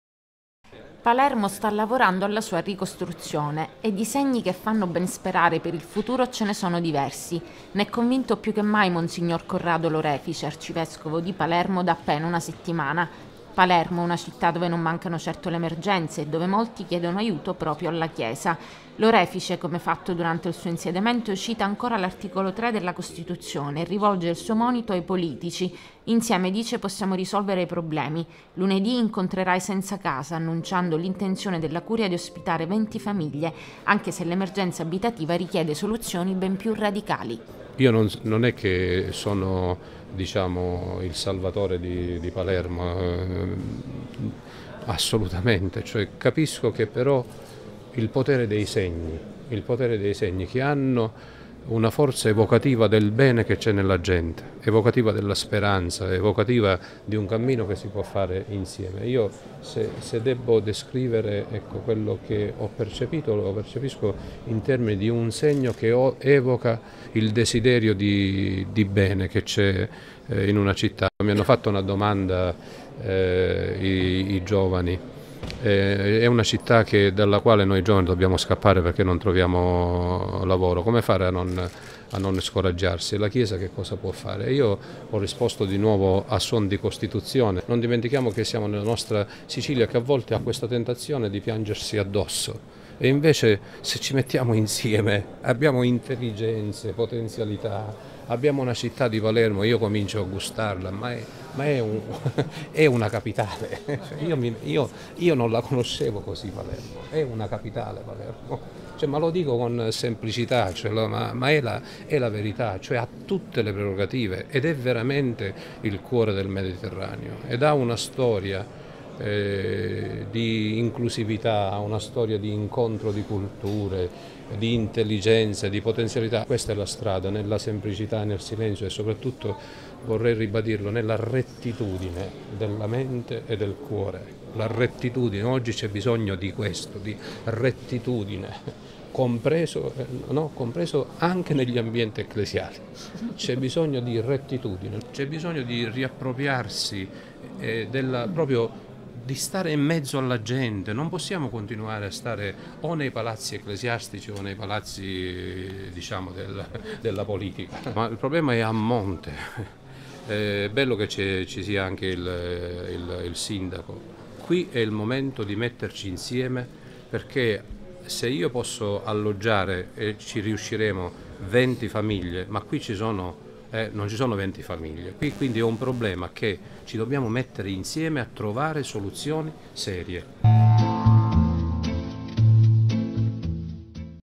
Il vescovo di Palermo: "Ecco come risolvere le emergenze della città"
Una città, Palermo, che per Lorefice “è una vera e propria capitale, fulcro del Mediterraneo, con innumerevoli potenzialità, energie e una storia di inclusività non indifferente”.